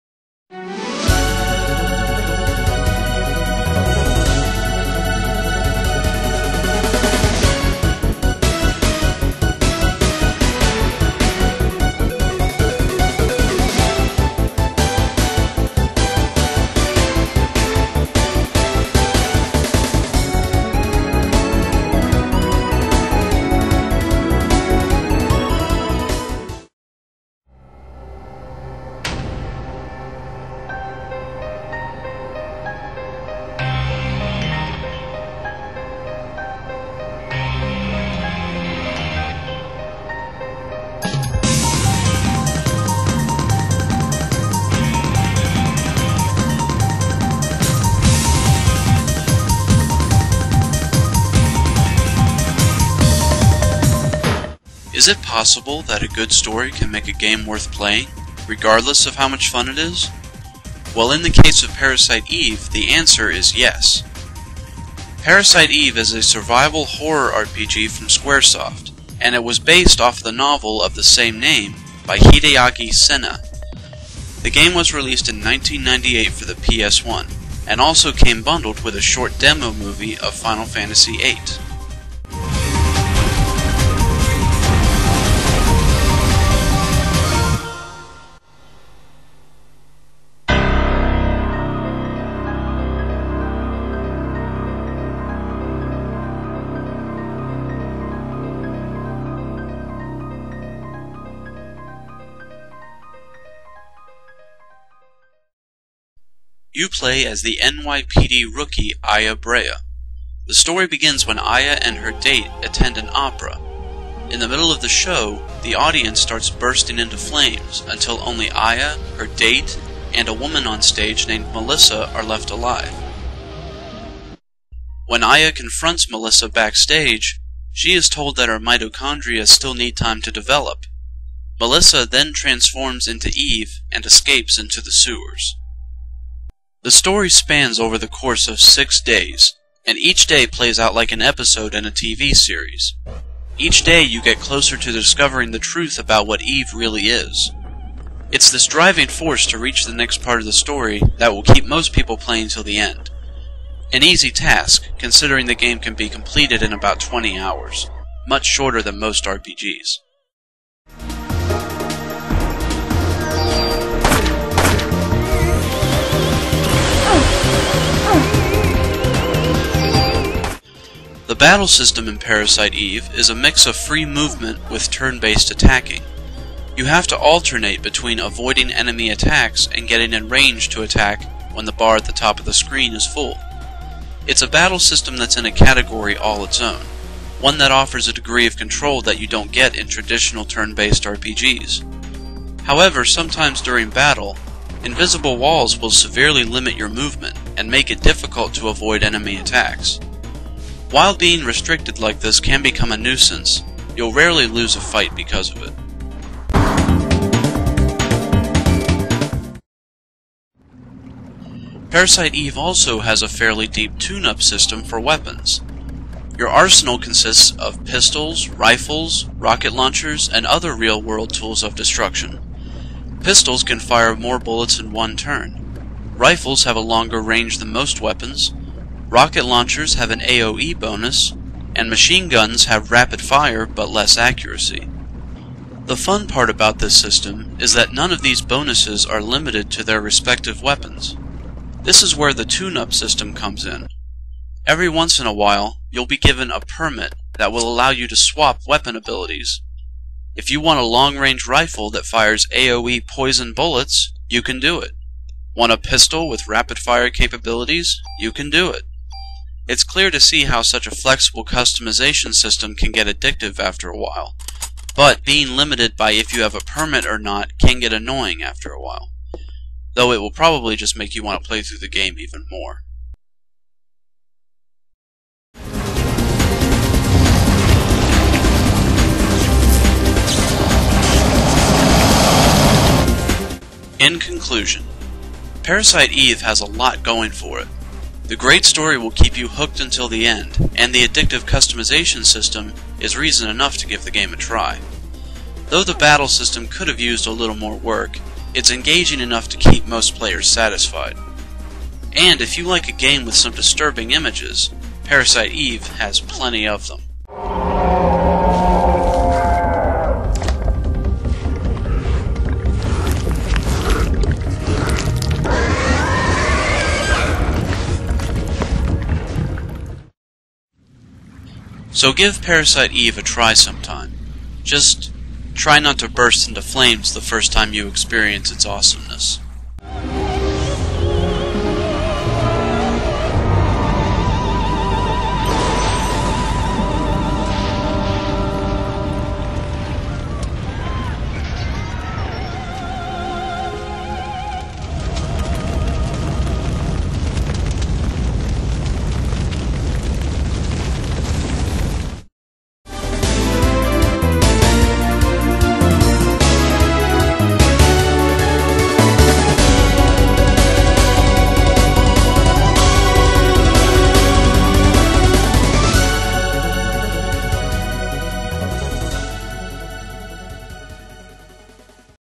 O.G.A.P. - Parasite Eve
Sorry for the extra wait on this one. Parasite Eve is a pretty good survival horror RPG from Squaresoft. Unfortunatly it was made when Square was still getting the hang of the Playstation hardware. The CG cutscenes show it. Nevertheless, it's a classic that should be given a try eventually.